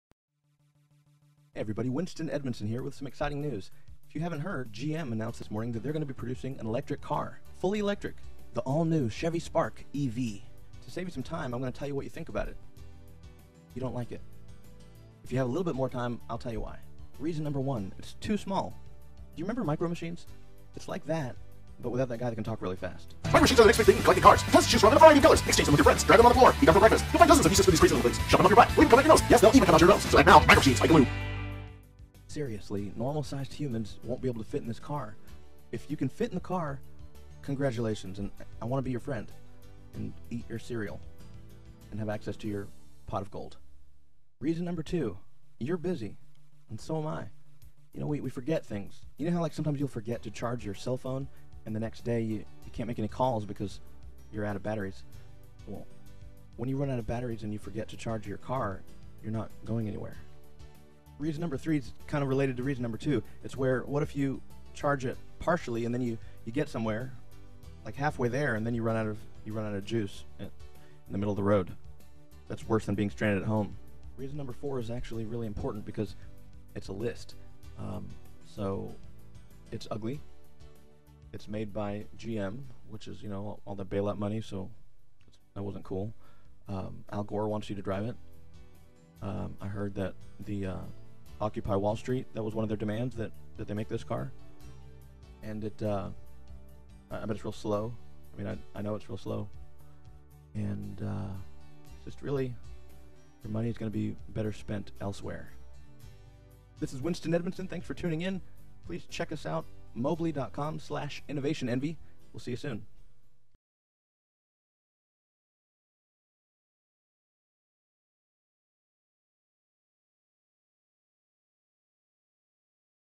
Exclusive First Look! Chevy Spark EV - Just Announced Fully Electric Vehicle via
Are you ready to go green?  I'm not talking lime green, I'm talking forest green.  Like, you might confuse it for black, green. You are!?  Then I guess you'll be driving the just announced Chevy Spark EV.  You've been whining, and GM's been listening.  Here's the full vehicle preview with expert analysis.  Like this video? Wanna see my secret stash outside of YouTube?  Come and get it!